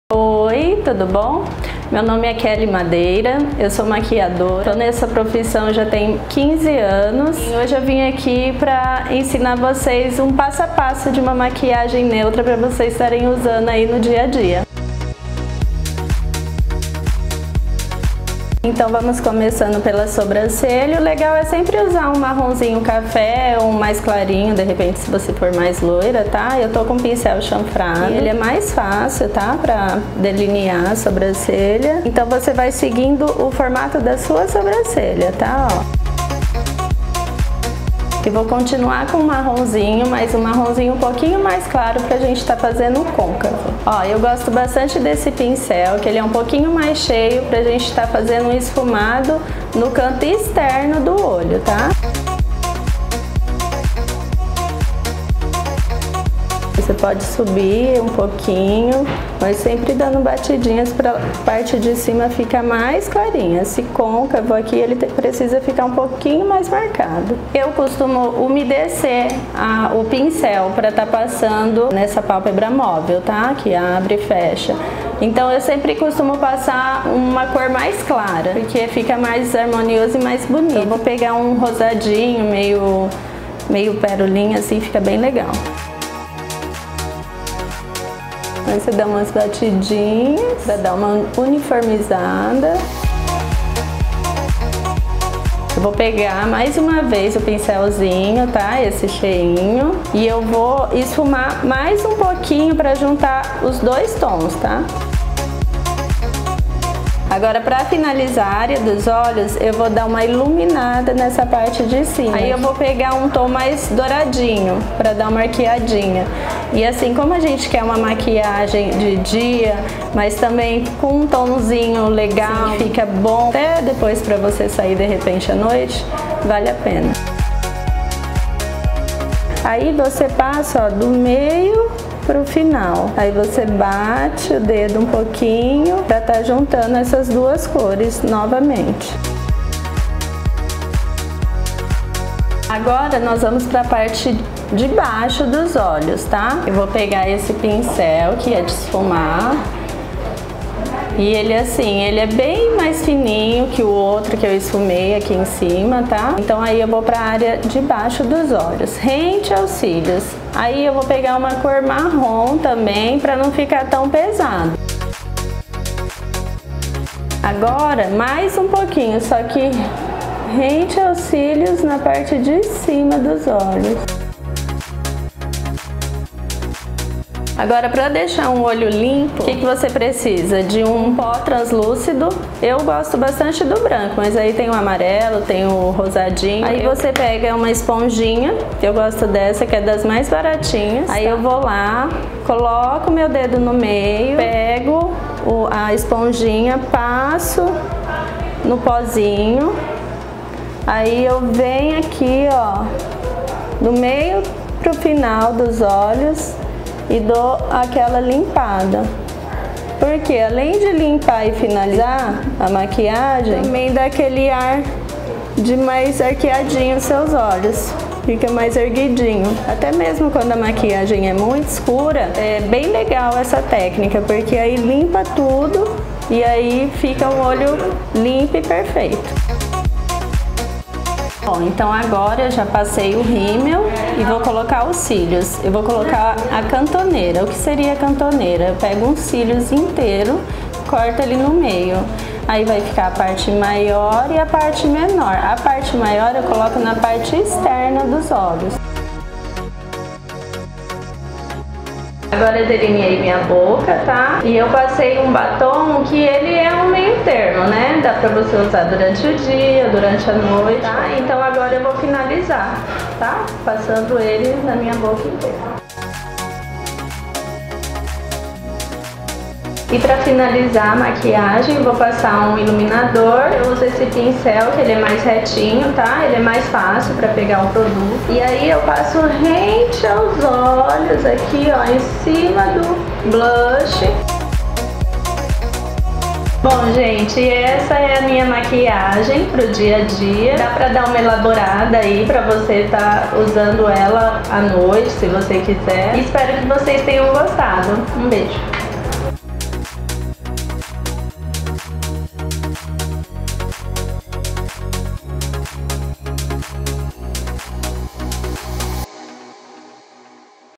MAQUIAGEM PARA O DIA A DIA
Uma maquiagem básica para o dia a dia, que pode ser usada em qualquer ocasião, é o que mais gostamos. E aprender como fazer de forma fácil e com as dicas de uma profissional, é melhor ainda.
Vocês votaram e hoje temos mais um tutorial com a make vencedora da enquete. Quer aprender como fazer essa maquiagem super linda e básica? Dá o play nesse vídeo tutorial que a Kelly Madeira te ensina as melhores técnicas.